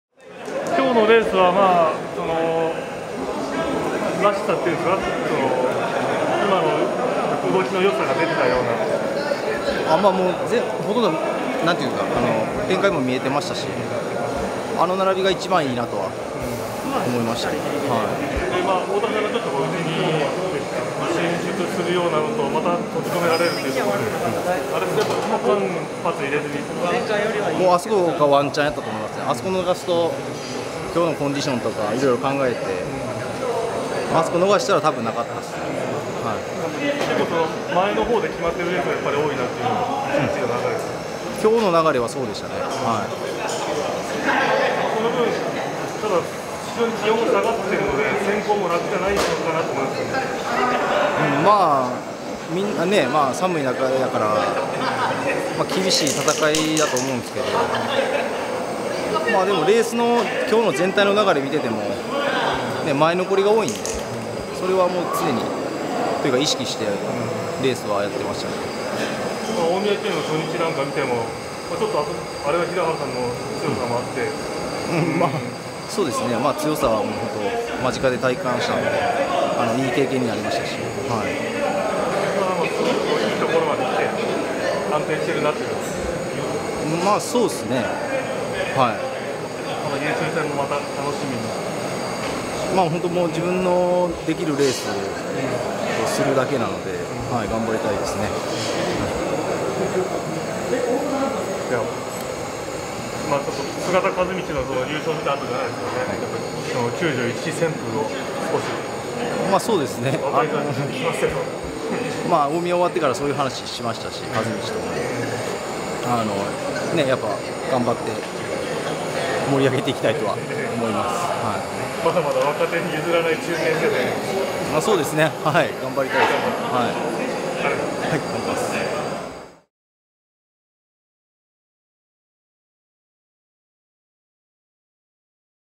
【GIII玉藻杯争覇戦】柴崎淳 小気味良いレース運びで２日目優秀戦へ
柴崎淳（３１）が高いレベルで安定している。初日特選１２Ｒを小気味良いレース運びで打開。近藤龍徳（２７）と２人で優秀「チータカカップ」に駒を進めた。ＧⅠ制覇の期待がかかる男が、ついに目覚めている。